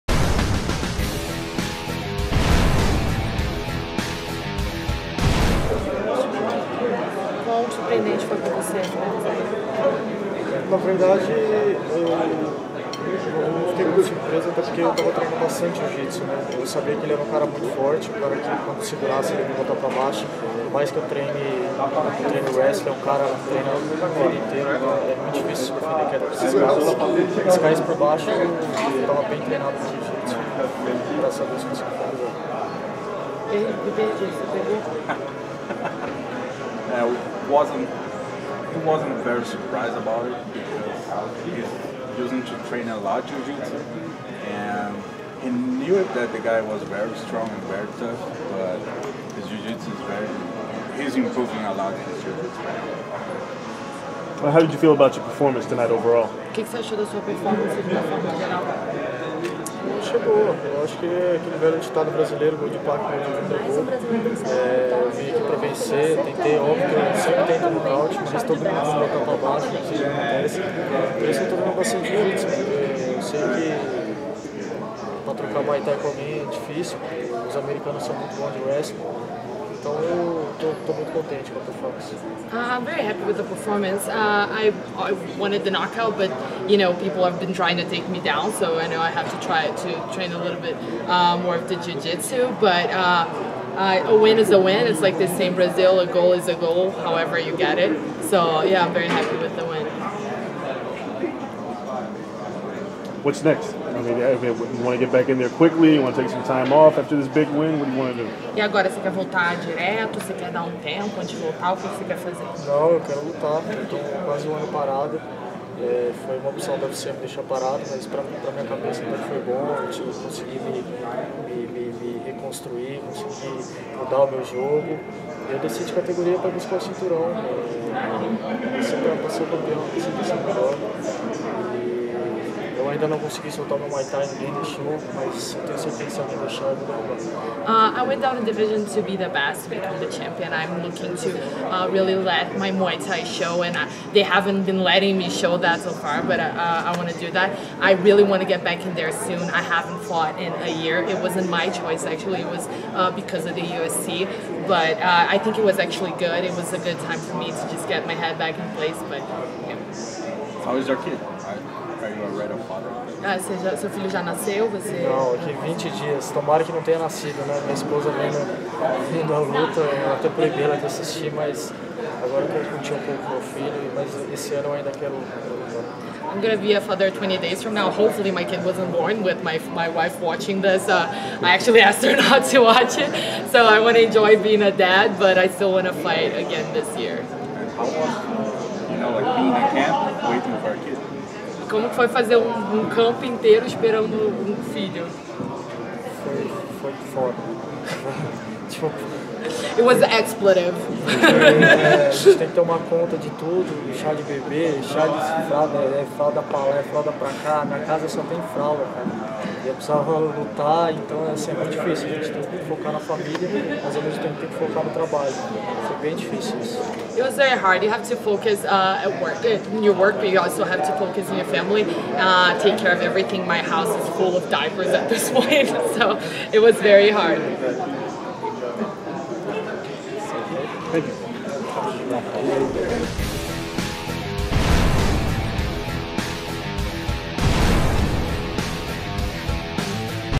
UFC Fight Night 90: Felipe Arantes Post Fight Interview
UFC bantamweight Felipe Arantes speaks with reporters backstage following his second round submission win over Jerrod Sanders at UFC Fight Night 90, which took place on July 7 at MGM Grand Garden Arena in Las Vegas.